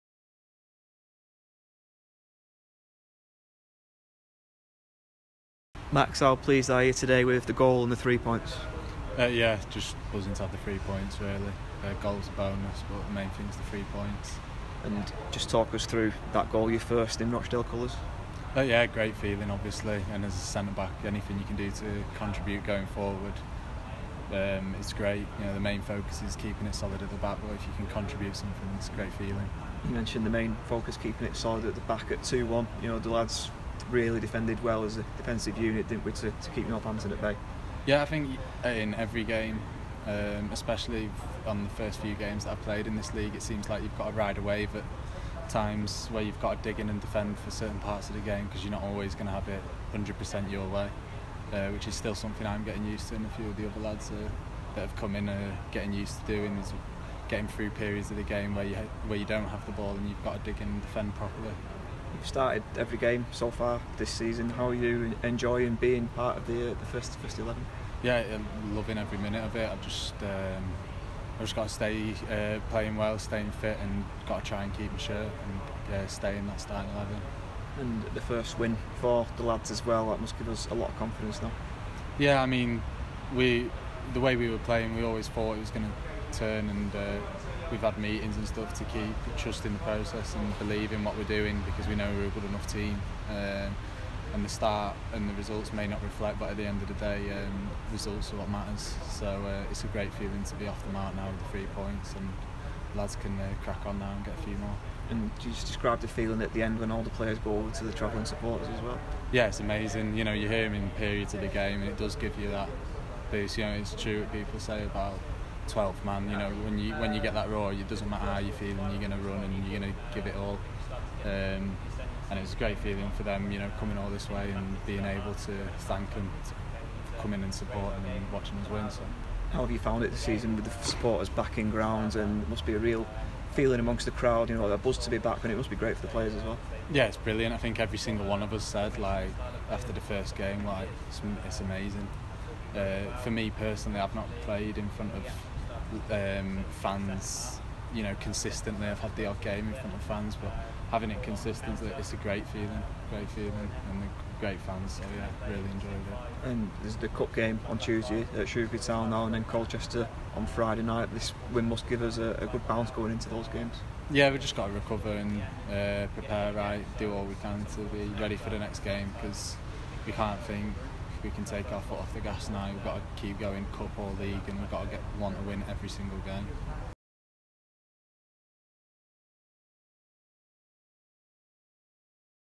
Max Taylor On Northampton Town Win
Max Taylor reflects on Dale's 3-1 win over Northampton Town at Sixfields Stadium.